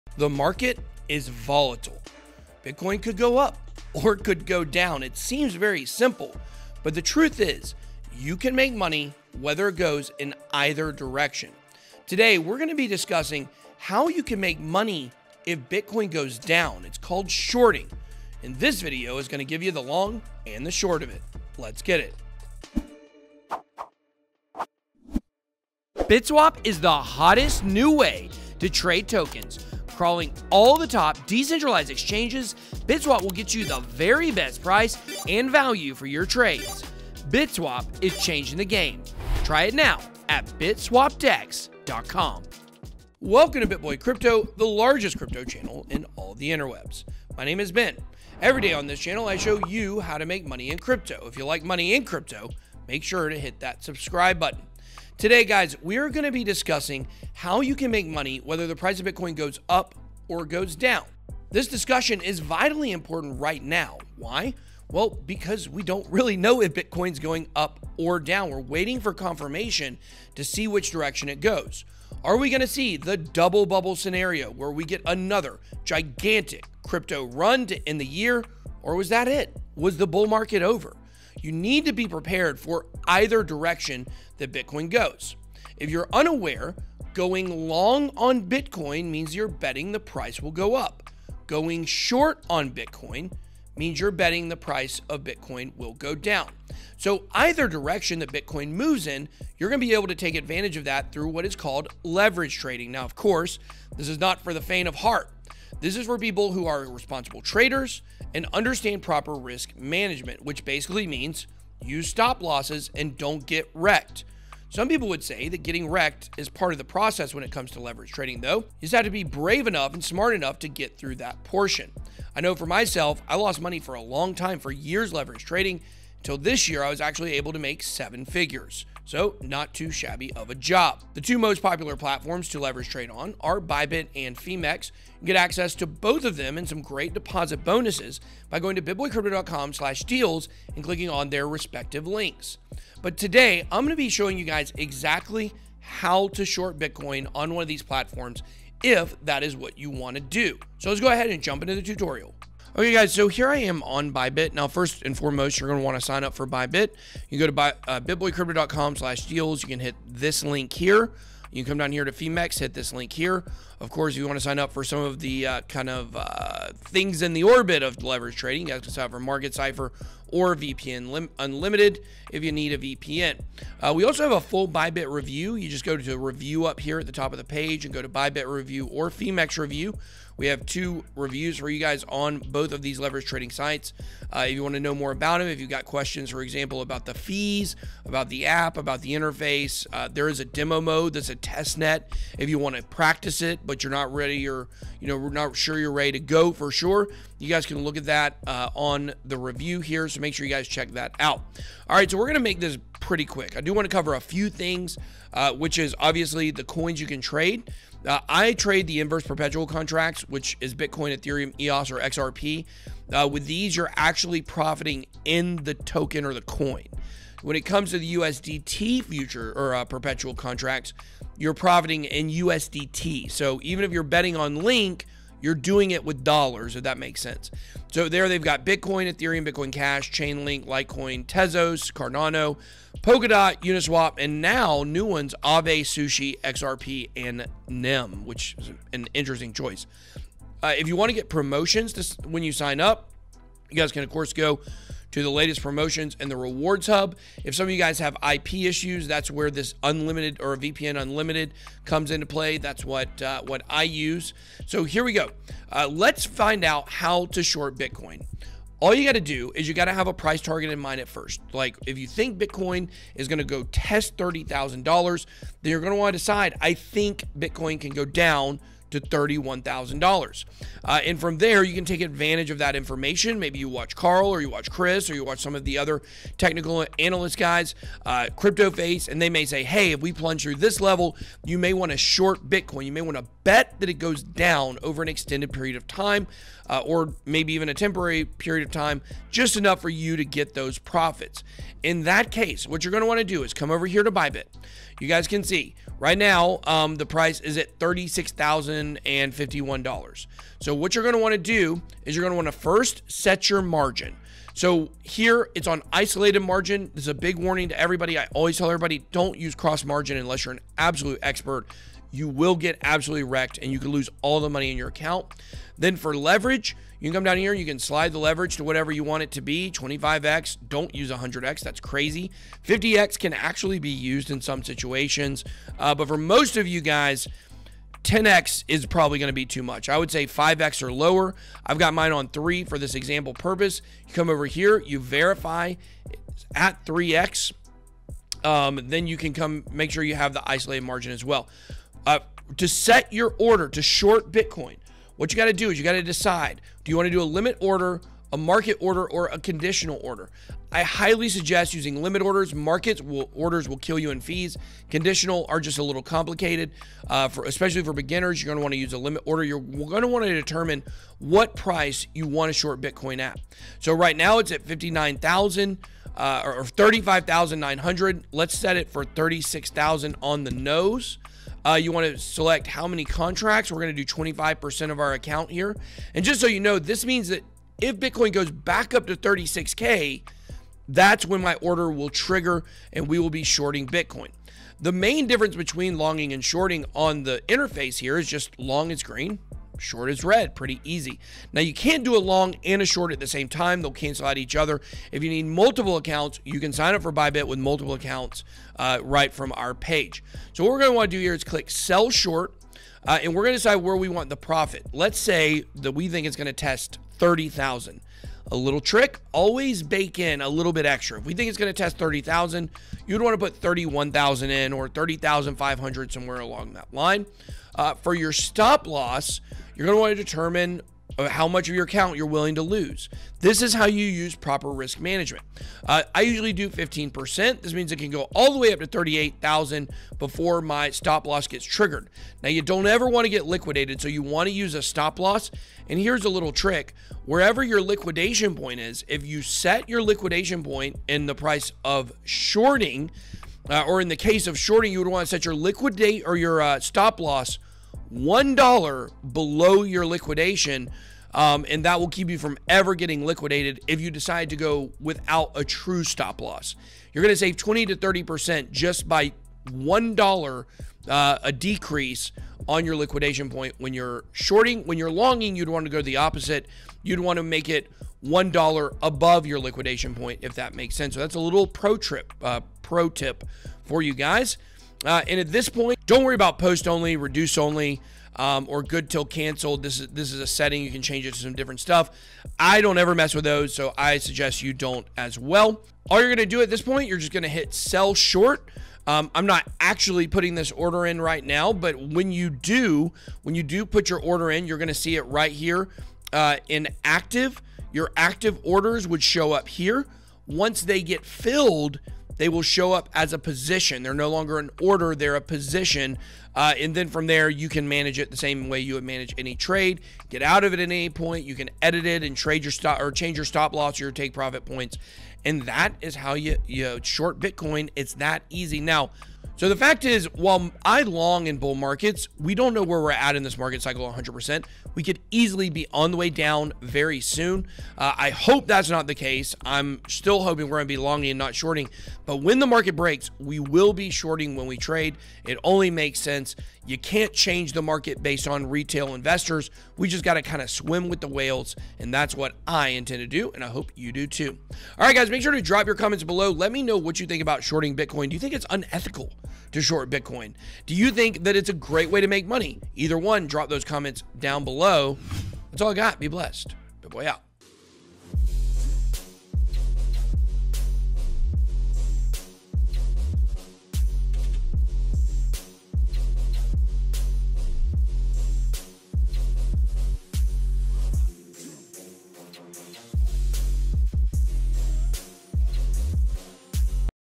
Perfect BTC Trading Scenario (How to Short Bitcoin Tutorial)
The market is volatile. Bitcoin could go up or down. You can make money whether it goes in either direction. We're going to be discussing how you can make money when Bitcoin goes down. It's called shorting Bitcoin.

In this video, I’ll show you how to short Bitcoin on ByBit. Shorting BTC can make you a lot of money if you correctly predict the price of Bitcoin dropping. Shorting cryptocurrency is a risky bet, so make sure you are prepared before you try it!

0:00 Intro
1:21 Going Long vs Going Short
2:11 Popular Leverage Trading Platforms
2:32 How to Short Bitcoin